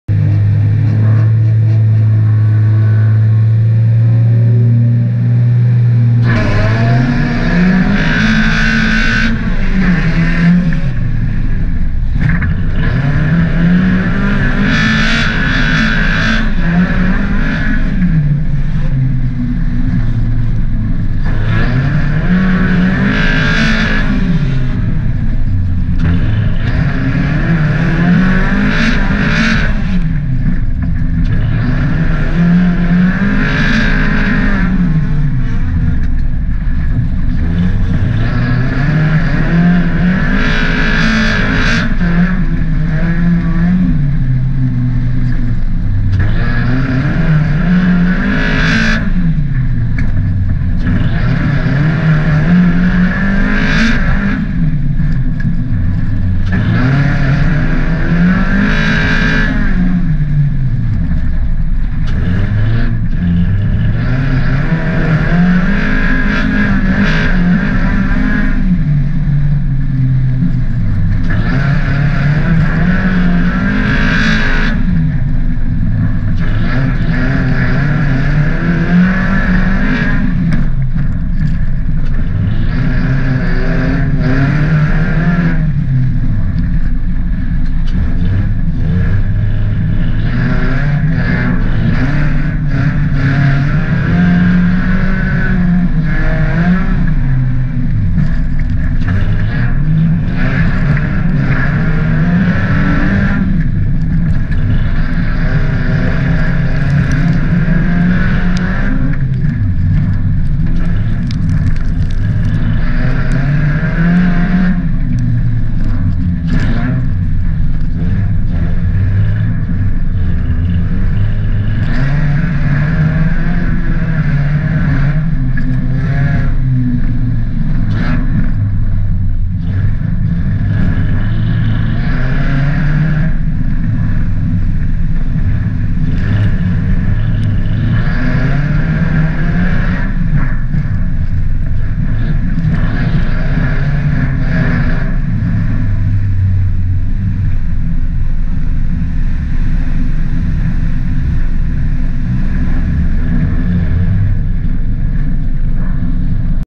Roncsderby 2018 - Rabatottos, Hungary - Heat 2
Driver: John Hajgato
Car: Mk3 VW Golf 1.8 20v
Placing 2nd in heat 2. (FR tire blow out in lap 2)